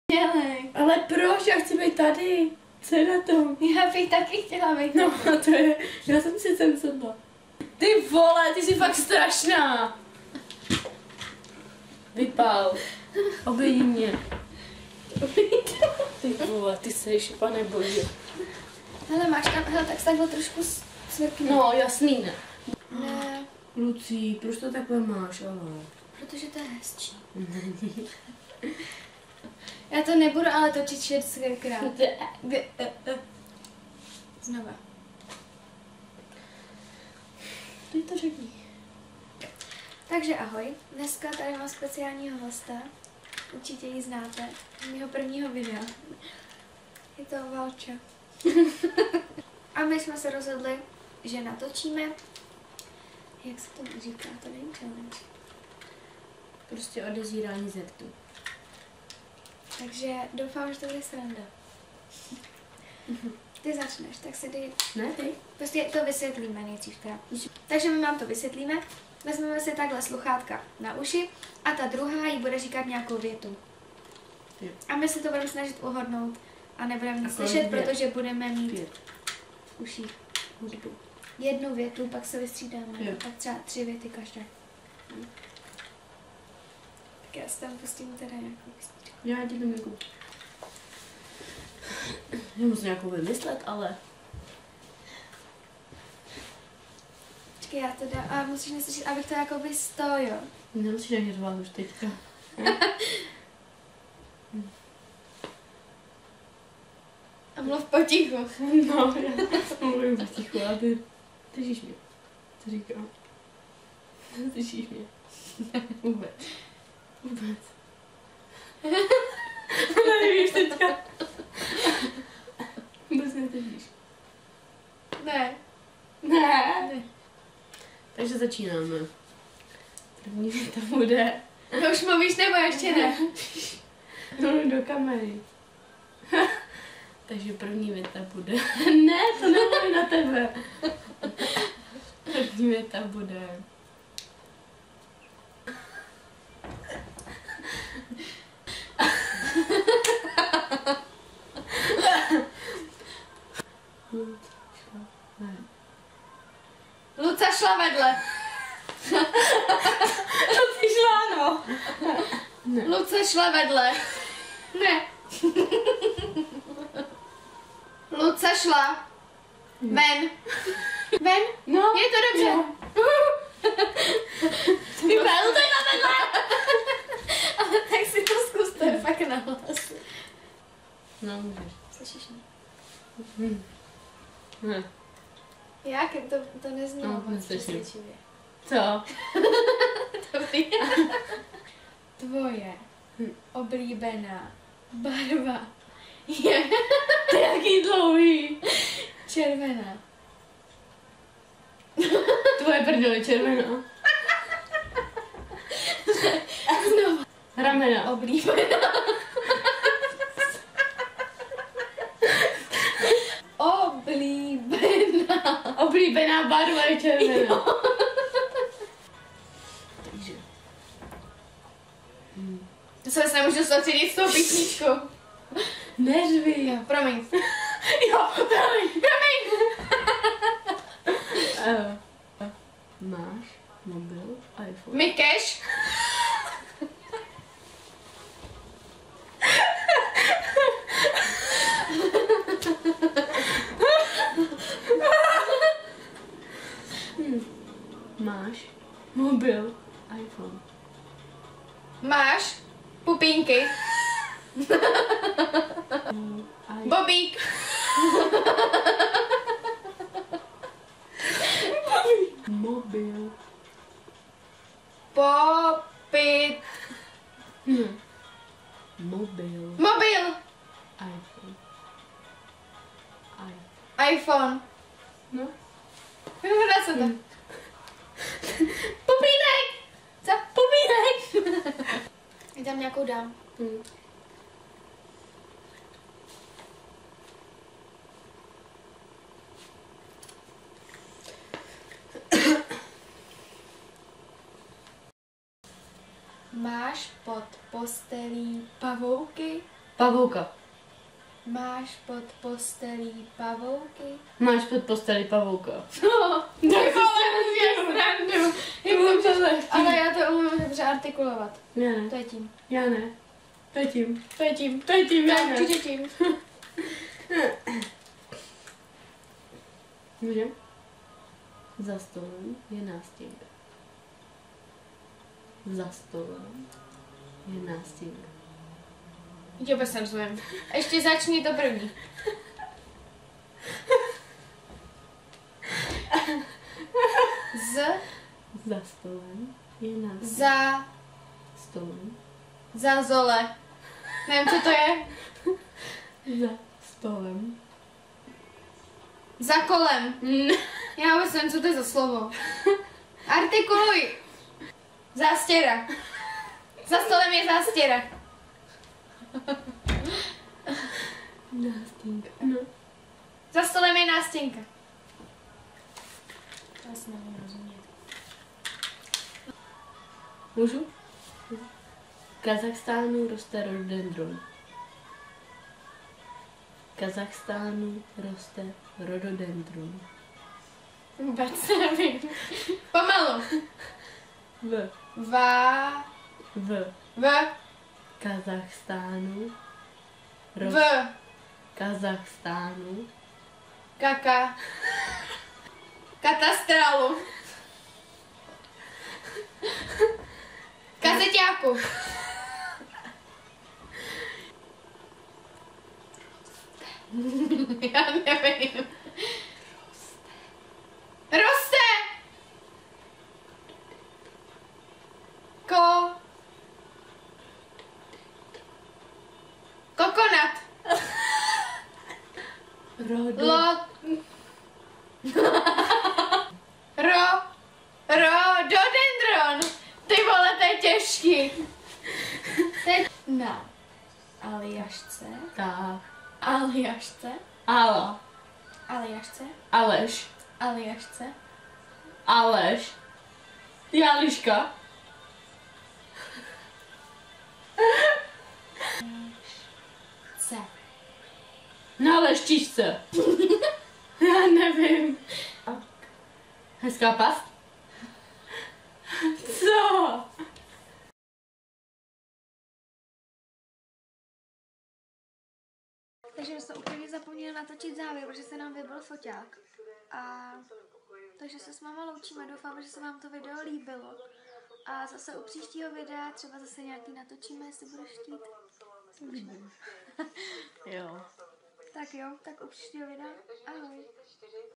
Odezírání ze rtů ft. Vali | Vynálezce nových slov
Zdravím vás s dalším videem! Tentokrát s mojí kamarádkou Valčou, kterou můžete znát z prvního vlogu.
Doufáme, že jste se pobavili stejně jako my :-D
Chcete další taková videa?